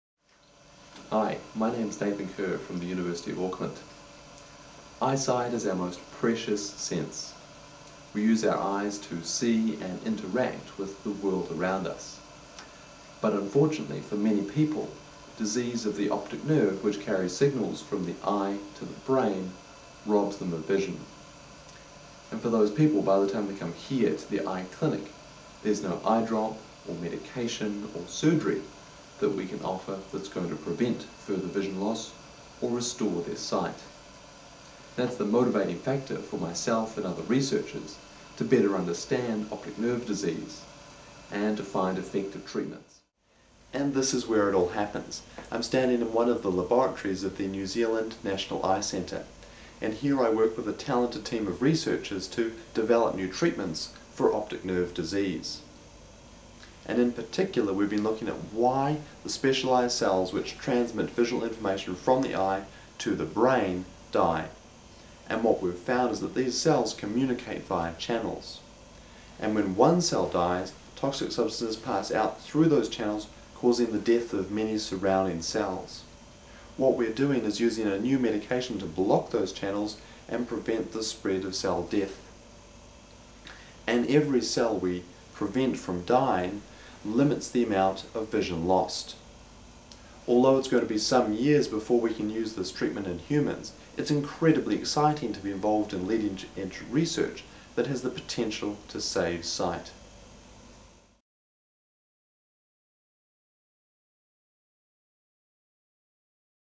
Nathan Kerr: Gap Junctions - Thieves of Sight
Video entries for the MacDiarmid Young Scientists of the Year Awards 2009. Video target audience is secondary schools.

Finalist: Advancing Human Health and Wellbeing